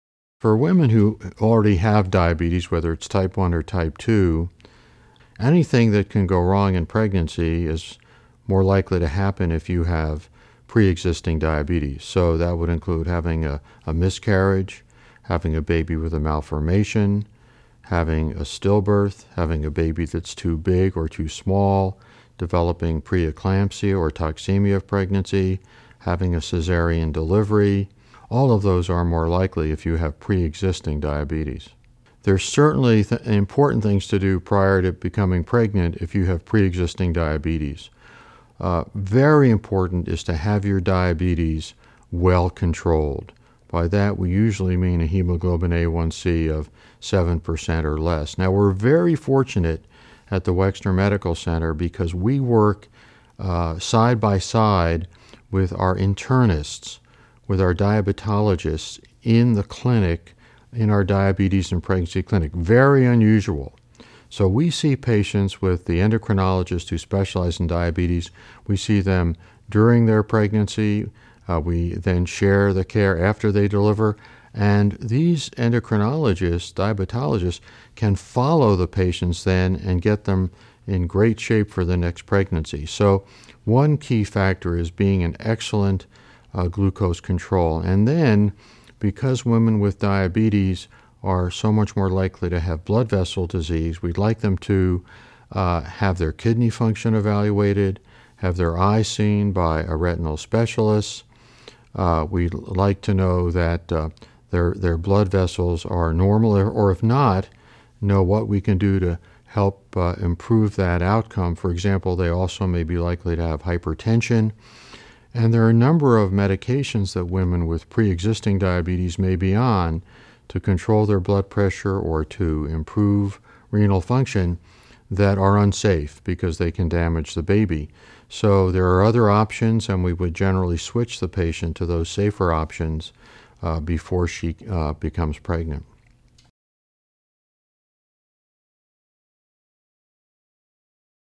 How Diabetics Should Prepare Prior to Pregnancy | Ohio State Medical Center
Women with existing Type 1 or Type 2 Diabetes face greater risk of pregnancy complications. For that reason, it is important for these women to prepare prior to becoming pregnant. As Steven Gabbe, MD, a Maternal Fetal Medicine physician at Ohio State’s Wexner Medical Center, explains, this includes having diabetes well under control, with a hemoglobin A1C of 7% or less, and checking overall blood vessel health, including a review of kidney function, eye health, and hypertension. In addition, some medications may need to be switched prior to becoming pregnant. Dr. Gabbe highlights Ohio State’s unique approach to diabetes care during pregnancy, with the Maternal Fetal Medicine specialists working closely with internists and diabetologists to ensure the best possible care.